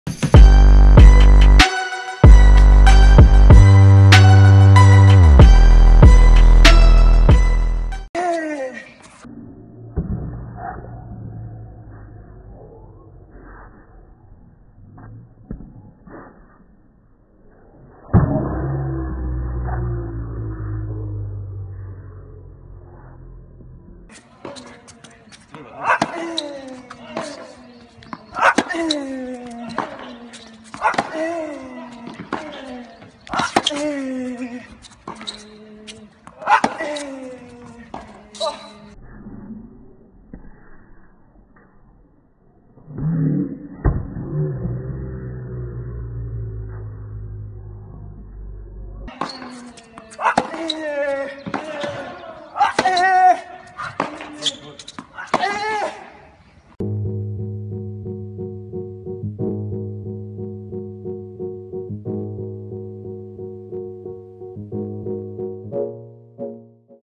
Dominic Thiem Backhand Slow Motion
Hope you enjoy one of the best one handed backhands in the game at the moment. See the evolution.
Muic: YouTubeAudioLibrary